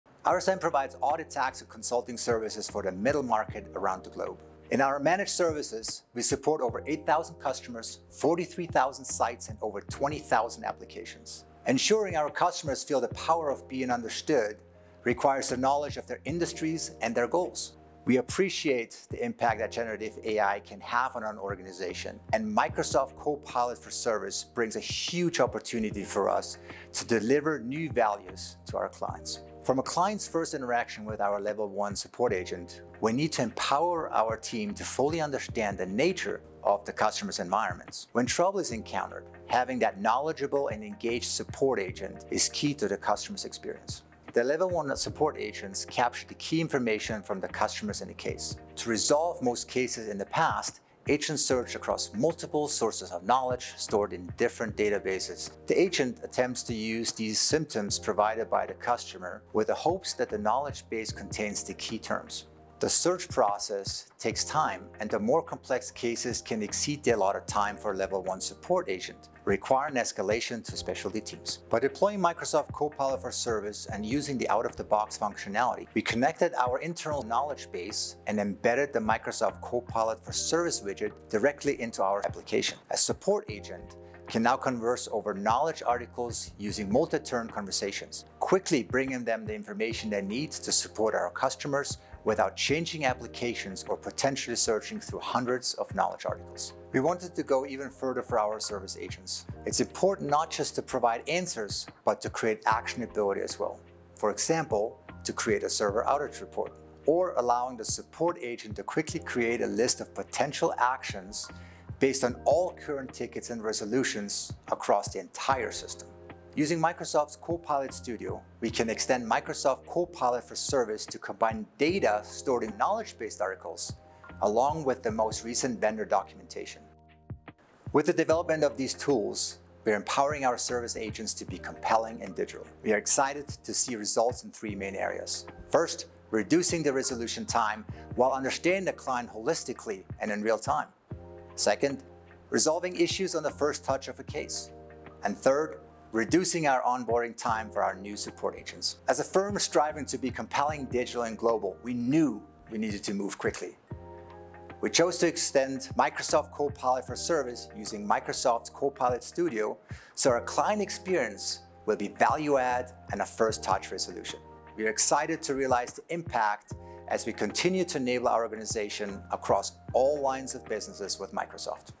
RSM deploys Microsoft Copilot for Service and Microsoft Copilot Studio to enhance customer service
RSM recently participated with Microsoft at Microsoft Ignite to showcase our latest innovation around AI with Copilot for Service and Copilot Studio.